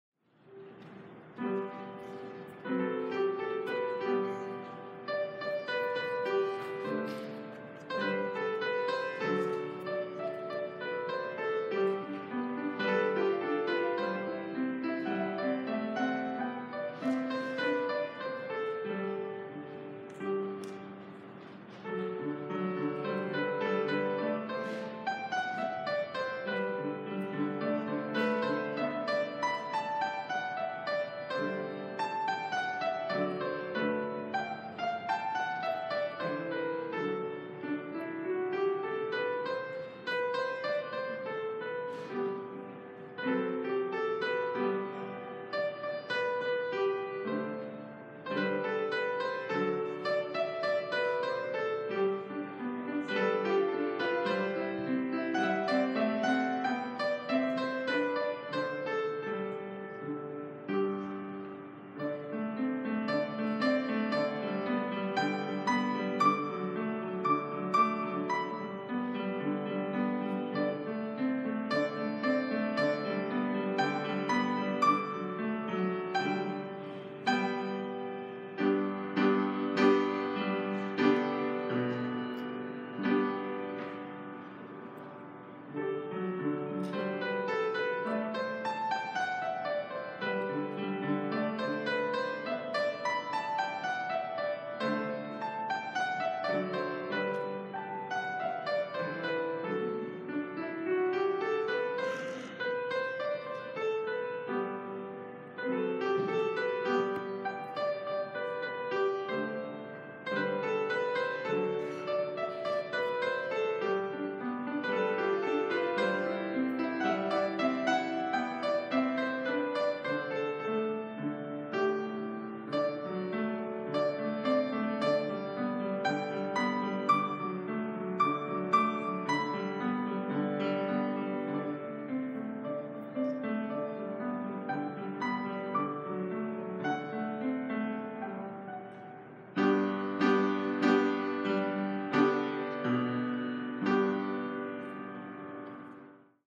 Special Music by Nora Zablow | July 15, 2018 |Trinity UMC Montpelier Vermont
Offertory Music @ Trinity UMC, Montpelier, Vermont